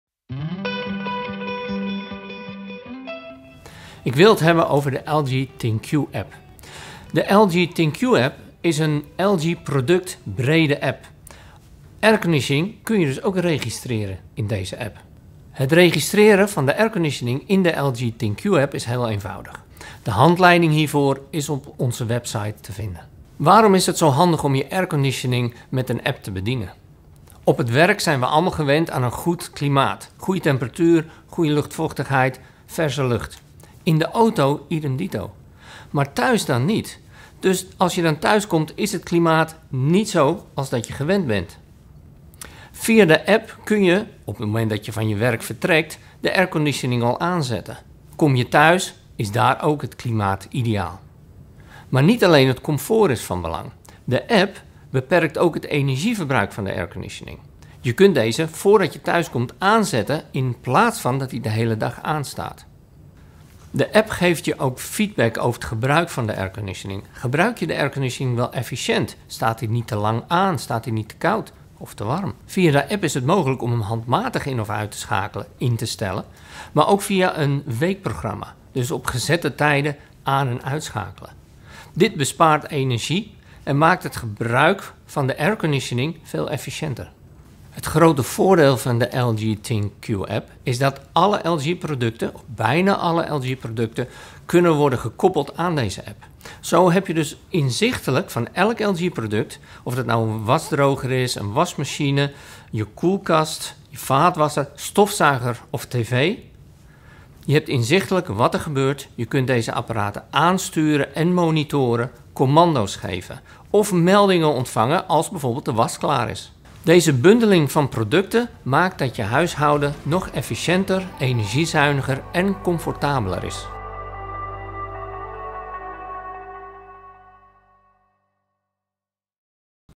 LG ThinQ App: alle voordelen op een rijtje
In deze video legt Service Manager Martin Pennings uit wat de voordelen zijn van spraakbesturing en energiemonitoring middels de LG ThinQ app.